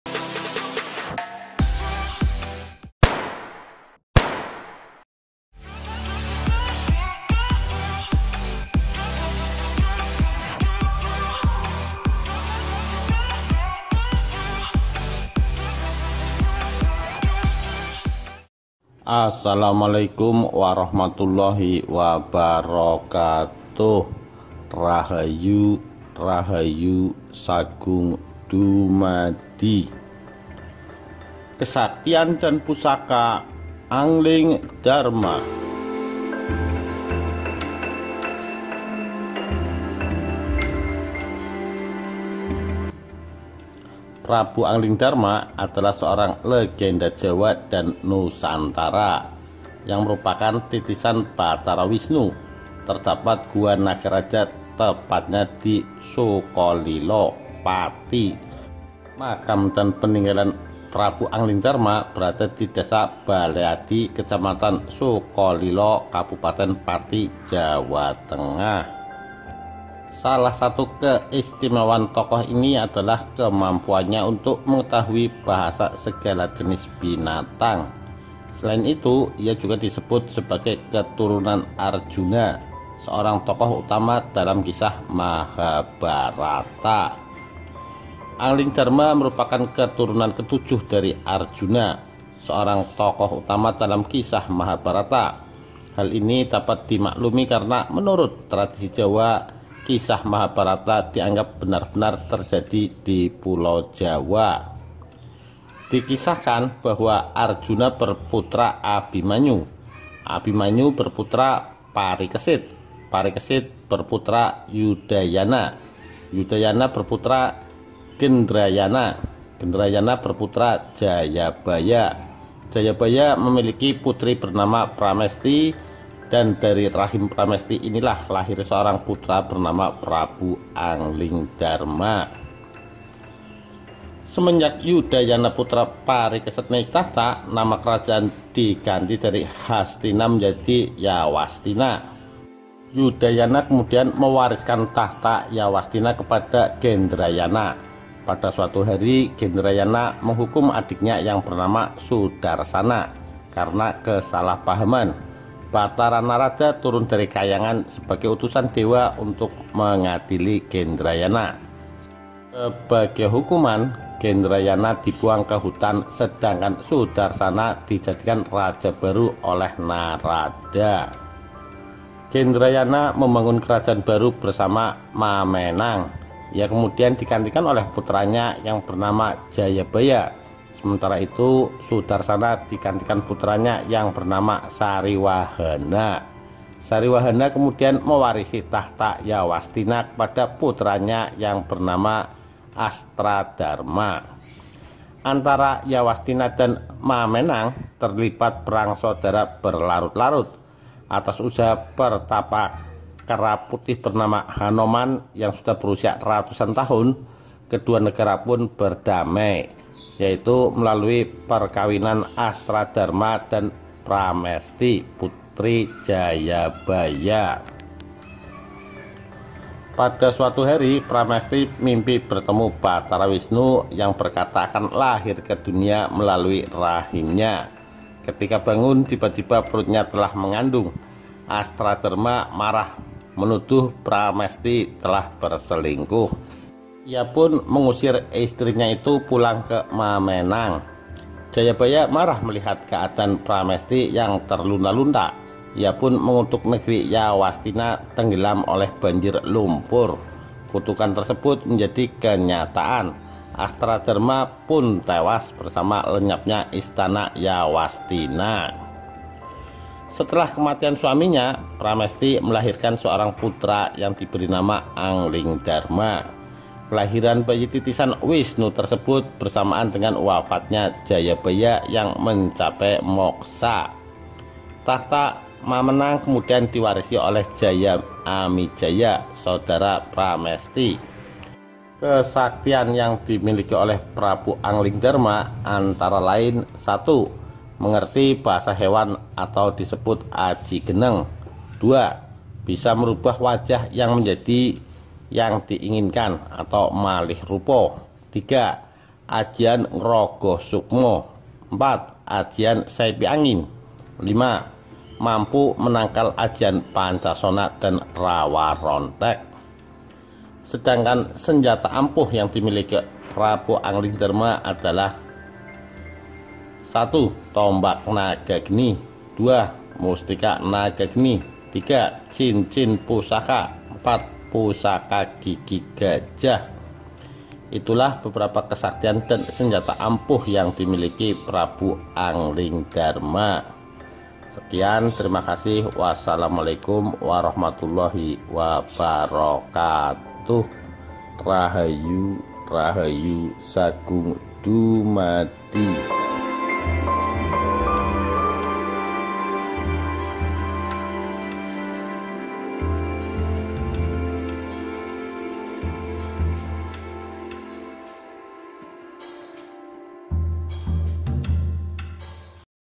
Kesaktian Dan Pusaka Angling Darma
Prabu Anglingdarma adalah seorang legenda Jawa dan Nusantara, yang merupakan titisan Batara Wisnu. Terdapat Gua NagaRaja tepatnya di Sukolilo, Pati. Makam dan Peninggalan Prabu Angling Dharma berada di Desa Baleadi, Kec. Sukolilo, Kab. Pati, Jawa Tengah.

Salah satu keistimewaan tokoh ini adalah kemampuannya untuk mengetahui bahasa segala jenis binatang. Selain itu, ia juga disebut sebagai keturunan Arjuna, seorang tokoh utama dalam kisah Mahabharata.

Anglingdarma merupakan keturunan ketujuh dari Arjuna, seorang tokoh utama dalam kisah Mahabharata. Hal ini dapat dimaklumi karena menurut tradisi Jawa, kisah Mahabharata dianggap benar-benar terjadi di Pulau Jawa.

Dikisahkan bahwa, Arjuna berputra Abimanyu. Abimanyu berputra Parikesit. Parikesit berputra Yudayana. Yudayana berputra Gendrayana. Gendrayana berputra Jayabaya. Jayabaya memiliki putri bernama Pramesti, dan dari rahim Pramesti inilah lahir seorang putra bernama Prabu Anglingdarma.

KELAHIRAN ANGLING DARMA

Semenjak Yudayana putra Parikesit naik takhta, nama kerajaan diganti dari Hastina menjadi Yawastina. Yudayana kemudian mewariskan takhta Yawastina kepada Gendrayana. Pada suatu hari Gendrayana menghukum adiknya yang bernama Sudarsana karena kesalahpahaman. Batara Narada turun dari kahyangan sebagai utusan dewata untuk mengadili Gendrayana. Sebagai hukuman, Gendrayana dibuang ke hutan sedangkan Sudarsana dijadikan raja baru oleh Narada.

Gendrayana membangun kerajaan baru bernama Mamenang. Ia kemudian digantikan oleh putranya yang bernama Jayabaya. Sementara itu, Sudarsana digantikan putranya yang bernama Sariwahana. Sariwahana kemudian mewariskan takhta Yawastina kepada putranya yang bernama Astradarma.

Antara Yawastina dan Mamenang terlibat perang saudara berlarut-larut. Atas usaha pertapa kera putih bernama Hanoman yang sudah berusia ratusan tahun, kedua negeri pun berdamai, yaitu melalui perkawinan Astradarma dengan Pramesti, putri Jayabaya.

Pada suatu hari Pramesti mimpi bertemu Batara Wisnu yang berkata akan lahir ke dunia melalui rahimnya. Ketika bangun tiba-tiba perutnya telah mengandung. Astradarma marah menuduh Pramesti telah berselingkuh. Ia pun mengusir istrinya itu pulang ke Mamenang.

Jayabaya marah melihat keadaan Pramesti yang terlunta-lunta. Ia pun mengutuk negeri Yawastina tenggelam oleh banjir lumpur. Kutukan tersebut menjadi kenyataan. Astradarma pun tewas bersama lenyapnya istana Yawastina.

Setelah kematian suaminya, Pramesti melahirkan seorang putra yang diberi nama Anglingdarma. Kelahiran bayi titisan Wisnu tersebut bersamaan dengan wafatnya Jayabaya yang mencapai moksa. Takhta Mamenang kemudian diwarisi oleh Jaya Amijaya, saudara Pramesti.